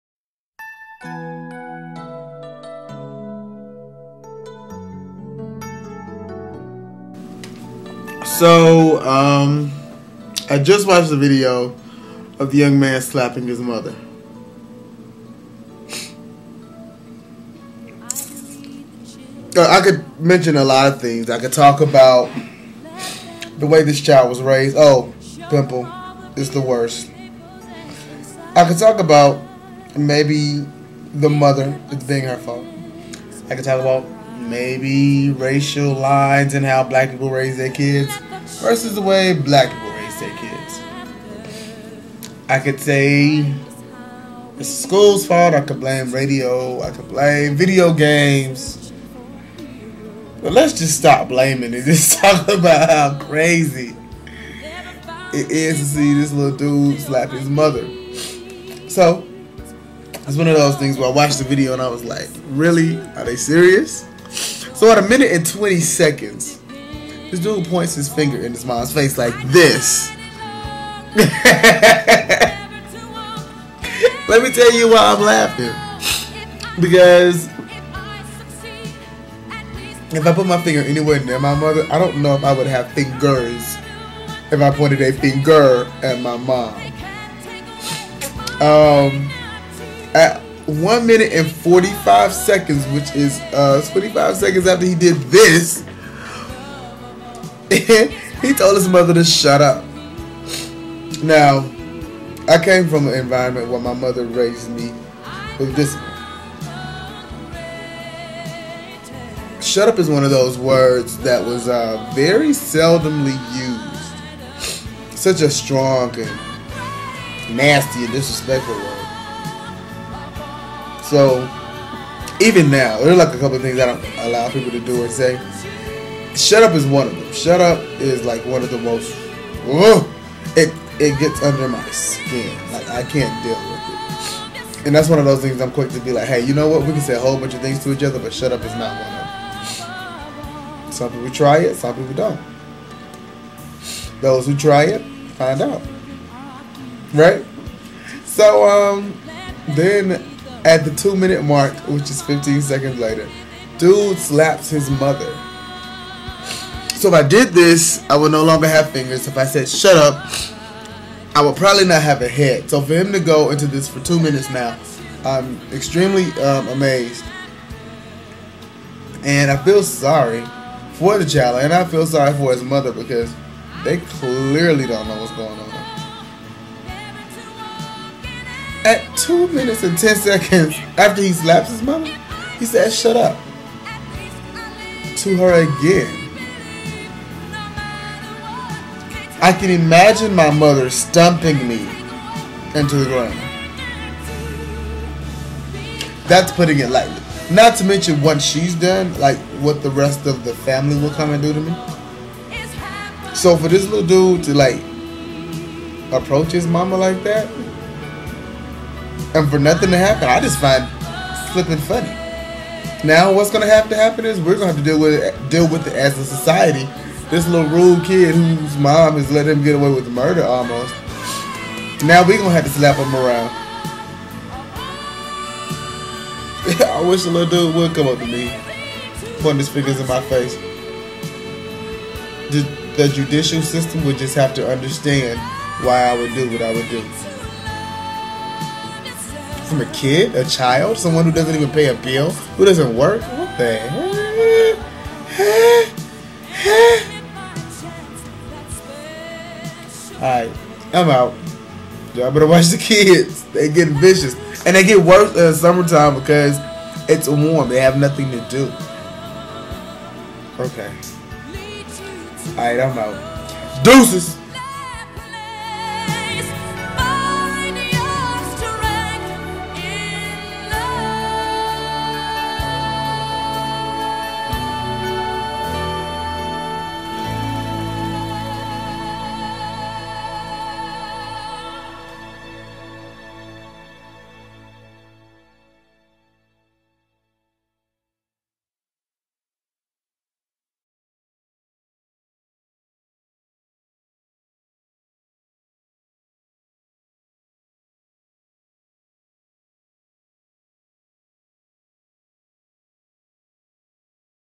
Son Slaps his mother....I wish I woulda...lol
My mama would kicked my everlasting...yall know what I mean...lol